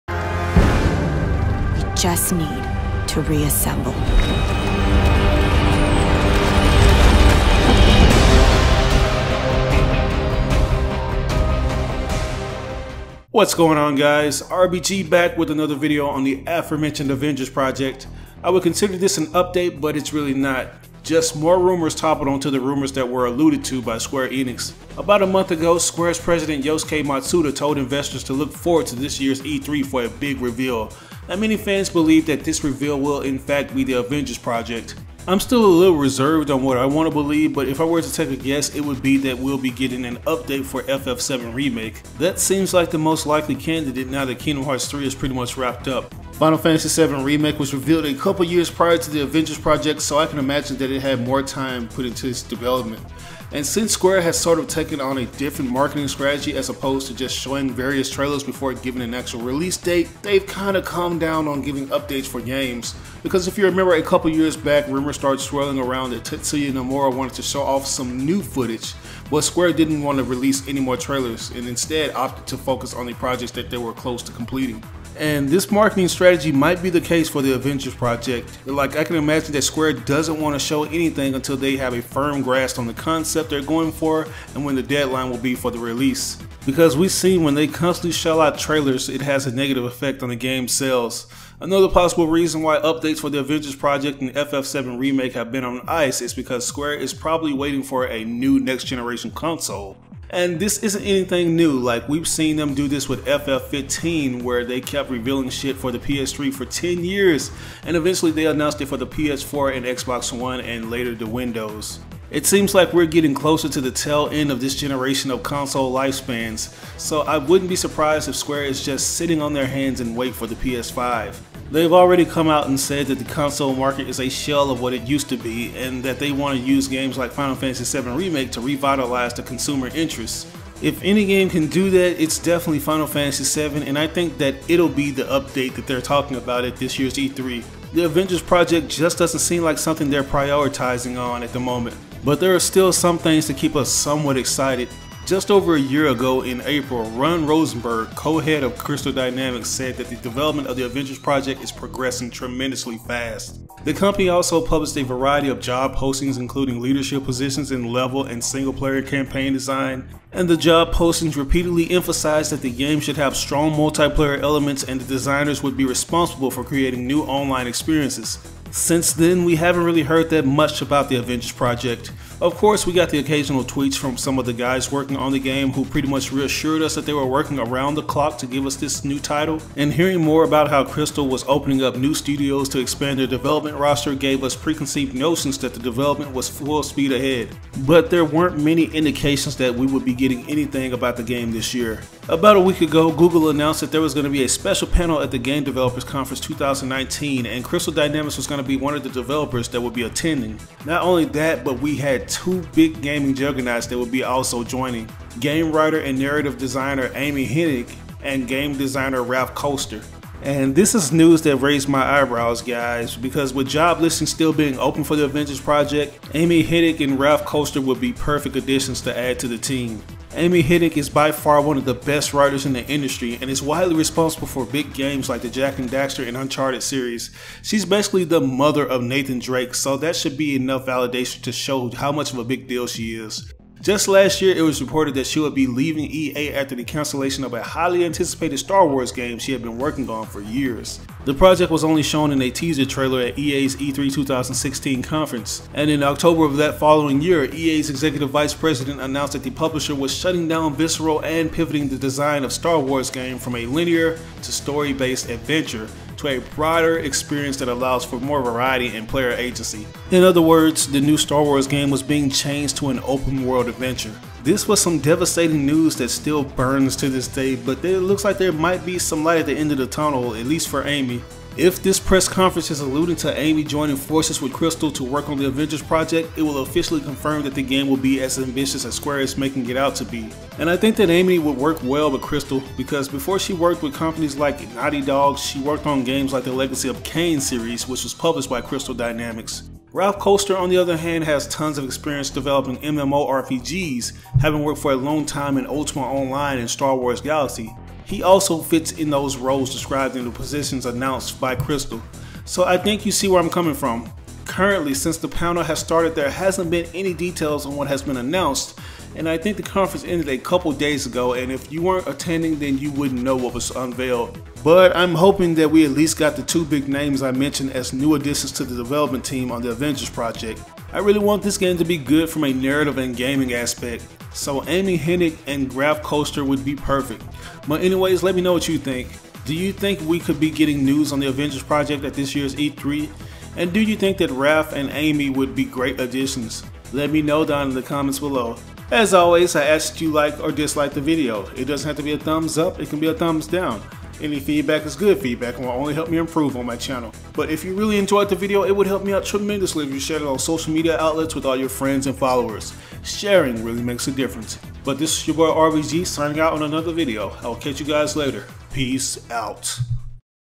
"New Avengers Game" | To be Released on Next Gen Consoles? | New Developers Hired
Avengers Project news, releasing on next gen consoles. More developers on board and close to completion?

I would consider this an update but it's really not. Just more rumors toppled onto other rumors that were aluded to by Square Enix. About a month ago Square's president Yosuke Matsuda told investors to look forward to this year's E3 for a big reveal. And many fans believe that this reveal will in fact be the Avengers Project. I'm still a little reserved on what to believe but if I were to take a guess it would be that we'll be getting an update to FF7 Remake. That seems like the most likely candidate now that Kingdom Hearts 3 is pretty much wrapped up. FF7 Remake was revealed a couple years prior to the Avengers Project so I can imagine that's had more time put into it's development. And since Square has sort of taken on a different marketing strategy as opposed to just showing various trailers before giving an actual release date. They've kinda calmed down on giving updates for games. Because if you remember a couple years back rumors started swirling around that Tetsuya Nomura wanted to show off some new footage. But Square didn't wanna release anymore trailers, instead opting to focus on projects that were close to completion. And this marketing strategy might be the case for the Avengers Project. Like I can imagine that Square doesn't wanna show anything until they have a firm grasp on the concept they're going for and when the deadline will be for the release. Because we've seen when they constantly shell out trailer it has a negative effect on a game's sales. Another possible reason why updates for the Avengers Project & FF7 Remake have been on ice is because Square is probably waiting to for a new next generation console.